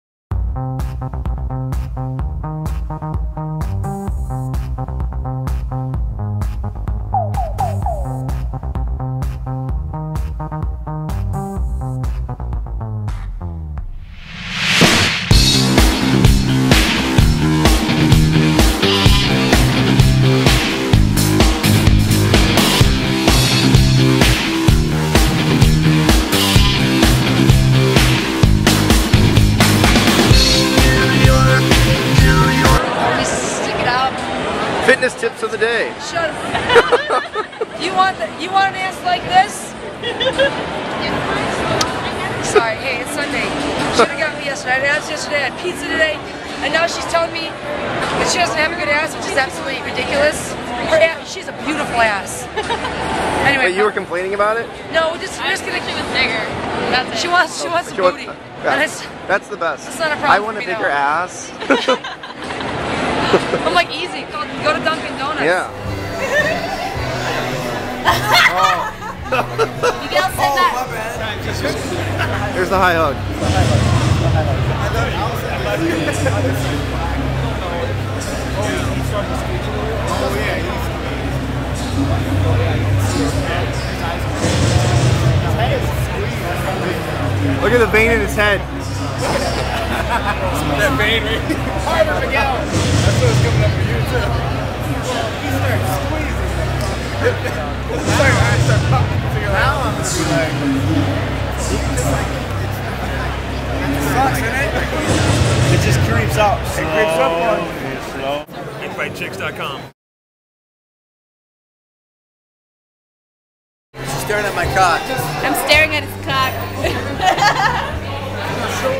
Fun Times with FIGHT CHIX
What does Miguel Torres, Arianny Celeste, Jen Widerstom, Jake Shields and Bas Rutten all have in common? They all love FIGHT CHIX. Meet the team Elisabeth, Jake, Darren, Miss RaRa and Trev Man. See Jen Widerstrom offer Arianny Celeste some fitness tips and learn the high hug from Miguel Torres, but don't stare!